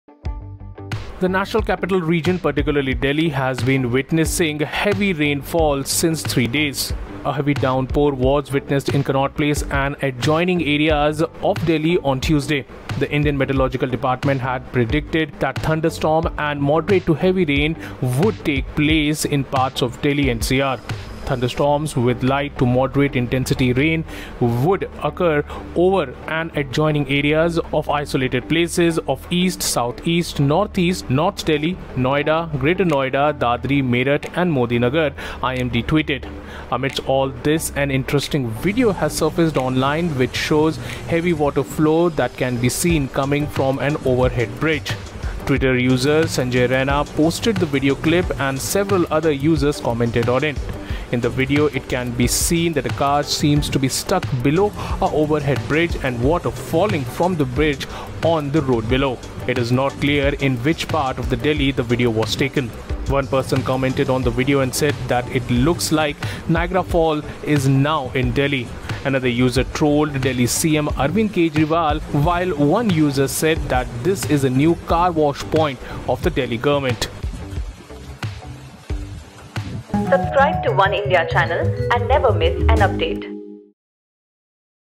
Delhi bridge over flows with water, user call it Niagara Fall | Oneindia News
A video of over flowing bridge in Delhi has gone viral, as people compare it to Niagara Fall. In the video it can be seen a car seems to be stuck below a over head bridge and water falling from the bridge on the road below.